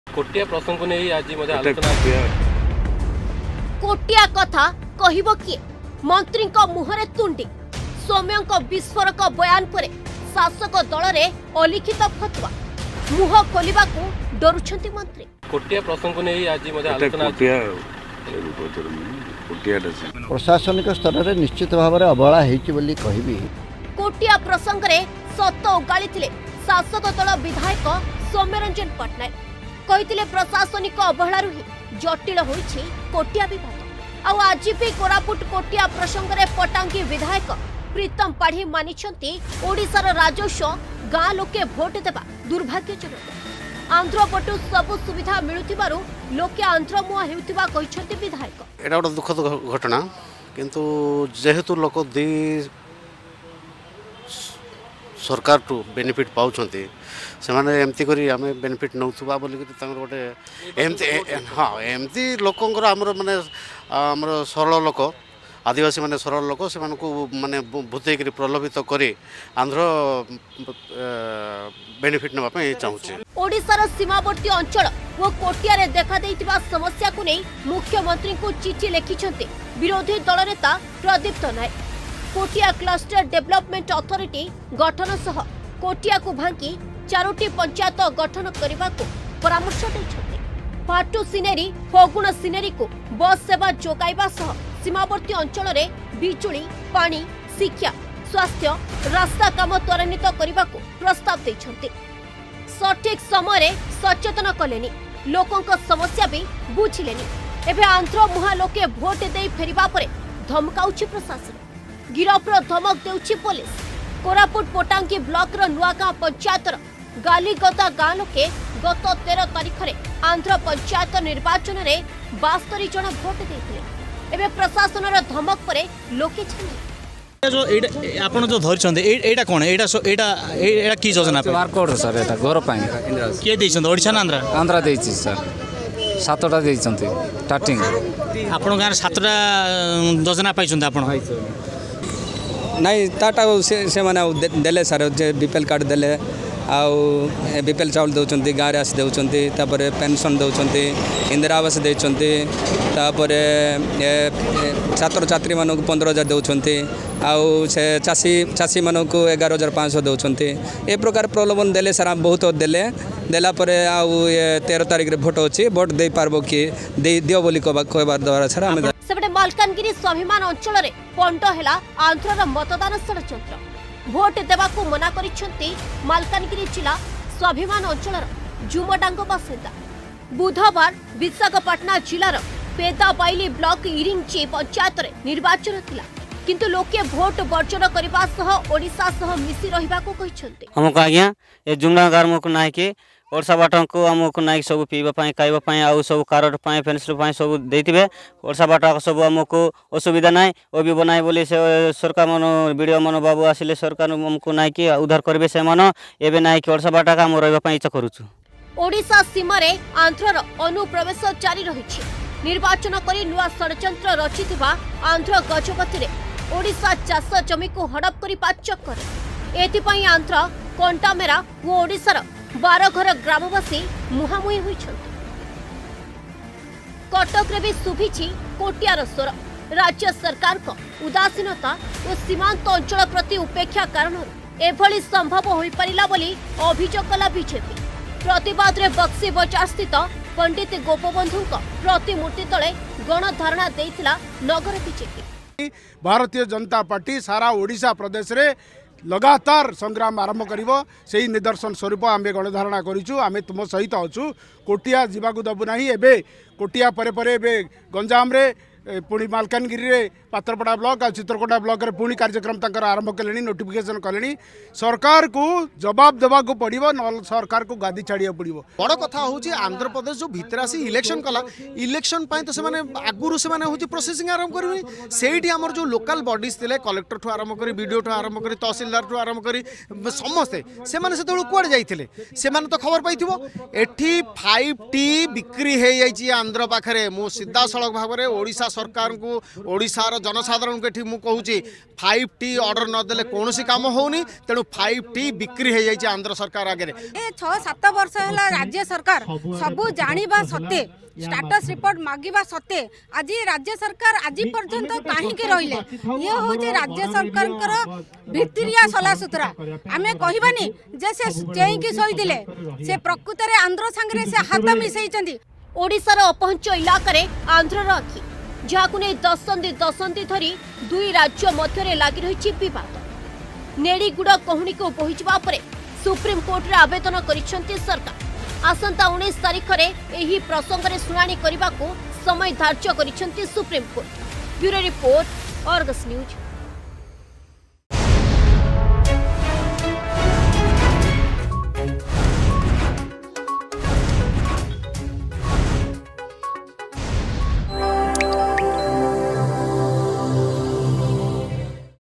କୋଟିଆ କଥା କହିବ କିଏ
କୋଟିଆକୁ ନେଇ ମନ୍ତ୍ରୀଙ୍କ ମୁହଁରେ ତୁଣ୍ଡି । ଆଜି ମନ୍ତ୍ରୀ ସୁଶାନ୍ତ ସିଂହଙ୍କୁ ପଚାରିବାରୁ ଉତ୍ତର ଦେଇ ପାରିନଥିଲେ ମନ୍ତ୍ରୀ । ଶାସକ ଦଳର ବିଧାୟକ ସୌମ୍ୟରଞ୍ଜନଙ୍କ ବିସ୍ଫୋରକ ବୟାନ ପରେ ବିଜେଡିରେ ଏବେ ଅଲିଖିତ ଫତୱା । ସେପଟେ ଏହି ପ୍ରସଙ୍ଗରେ ମୁଖ୍ୟମନ୍ତ୍ରୀଙ୍କୁ ଚିଠି ଲେଖିଛନ୍ତି ବିରୋଧୀ ଦଳ ନେତା ପ୍ରଦୀପ୍ତ ନାୟକ...